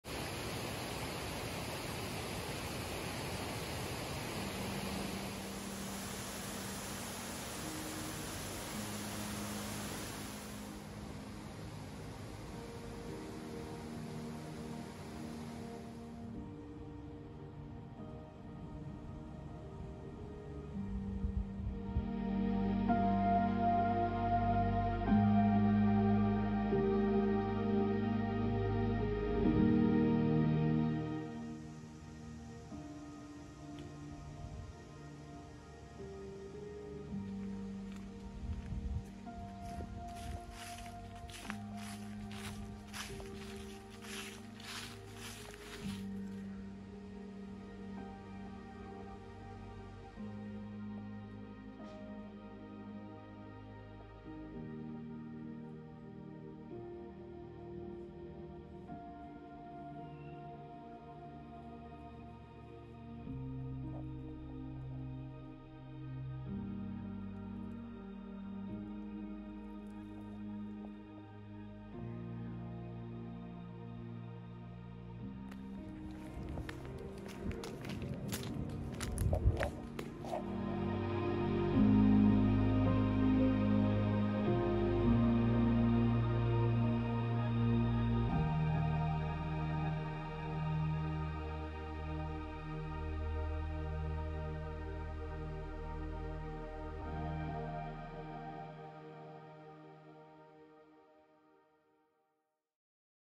Enjoy the peace and quiet of an evening walk in autumn 🍂 🌿🍁🍃🍂
Einer meiner Lieblingsplätze für einen schnellen Spaziergang oder eine Runde mit dem Fahrrad ist der Weg durch under Dorf über due Felder und am Fluss entlang nature 🍂🍃🍁🤗